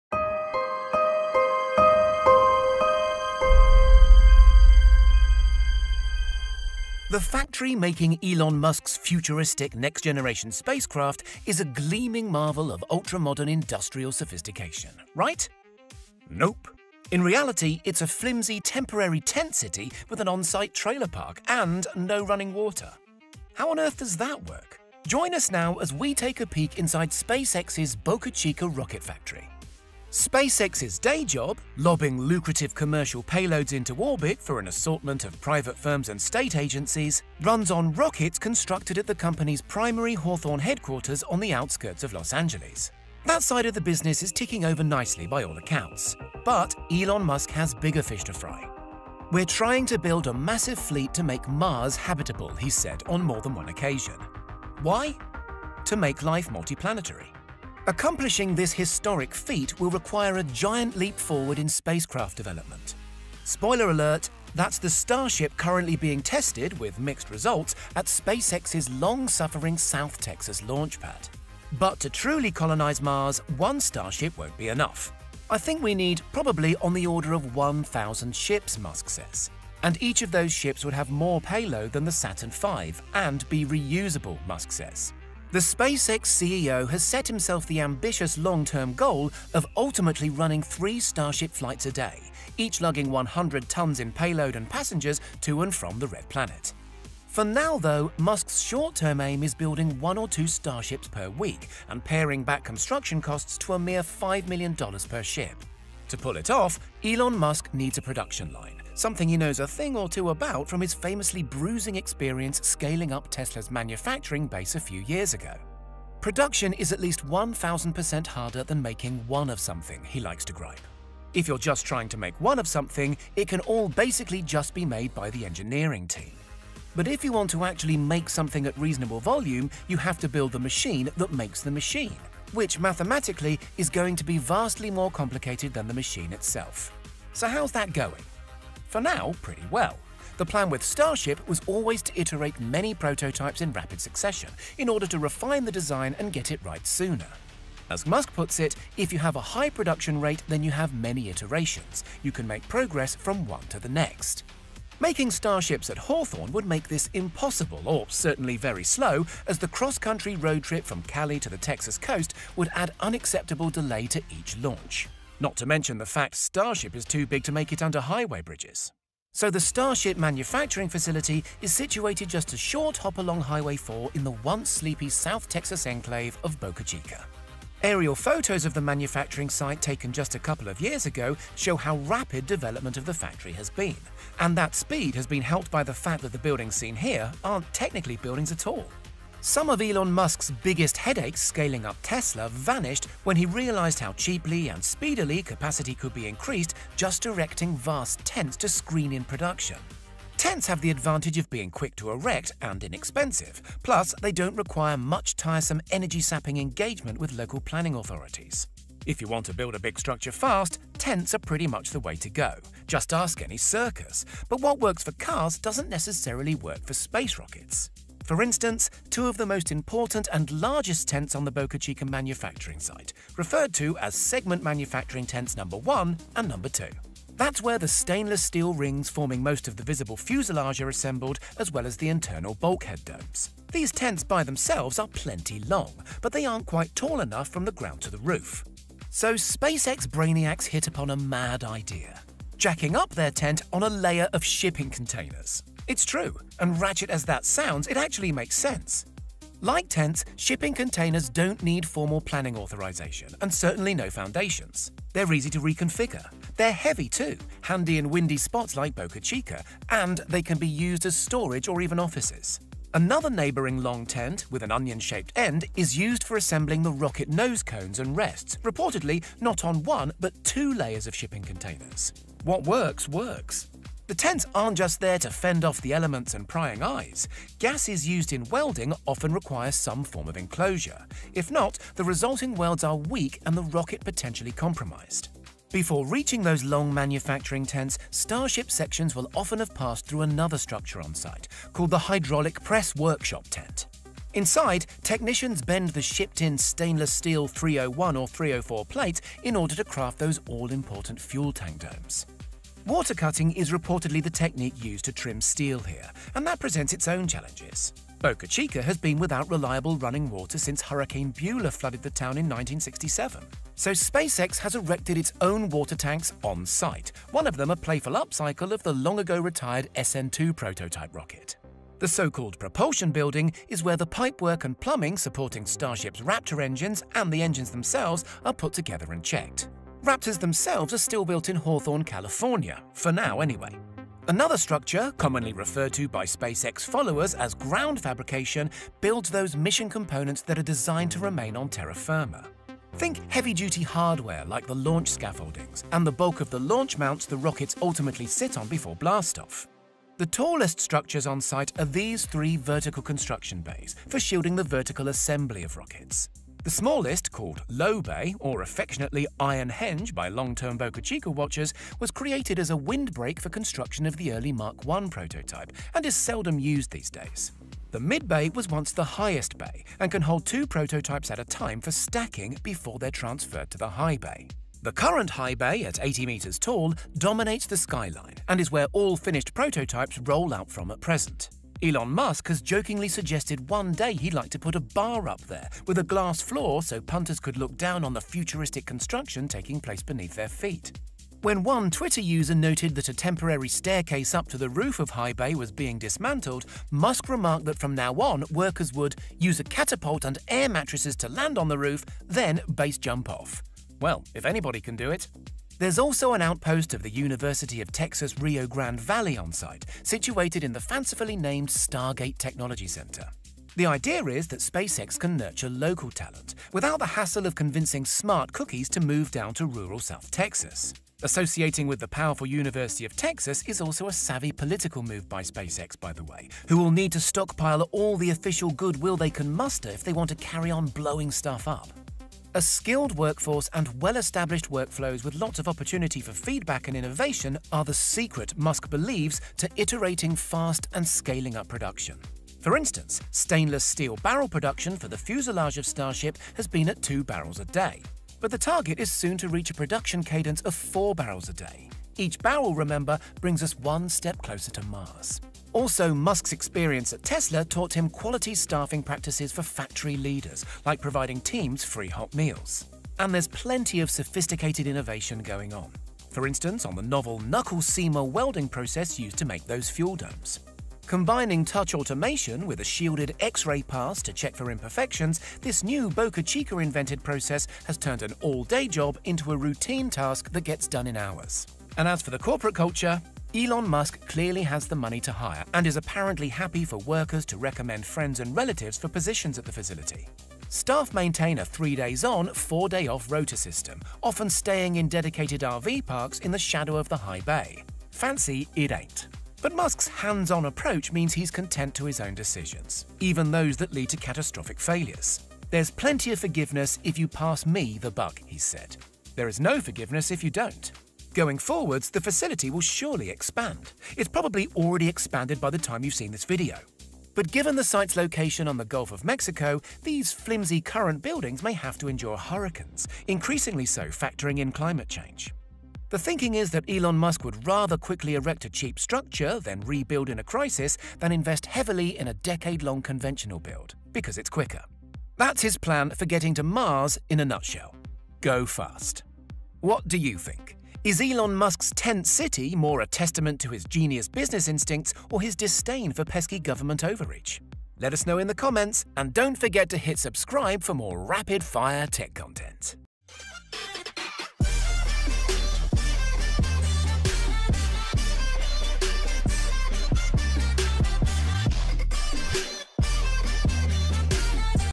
Inside SpaceX’s Texas Rocket Factory
We live in the future, because there's nowhere else to go.
Learn about the latest cool technology in only a couple minutes!

The factory making Elon Musk’s futuristic next-generation spacecraft is a gleaming marvel of ultra-modern industrial sophistication, right? Nope. In reality it’s a flimsy, temporary tent city, with an onsite trailer park – and no running water.

How on earth does that work? Join us now as we take a peek inside SpaceX’s Boca Chica rocket factory.

It’s mission? Nothing short of saving the world.
ll                     ll
ll                     ll
¯ \ _ (ツ) _ / ¯
SUGGEST A TOPIC 🚀🖆℁🎖😍🧐👇
save green save earth 🍃♻🎪🚩☝

Imagery supplied via Pixel Images

Inside SpaceX’s Rocket Factory In Texas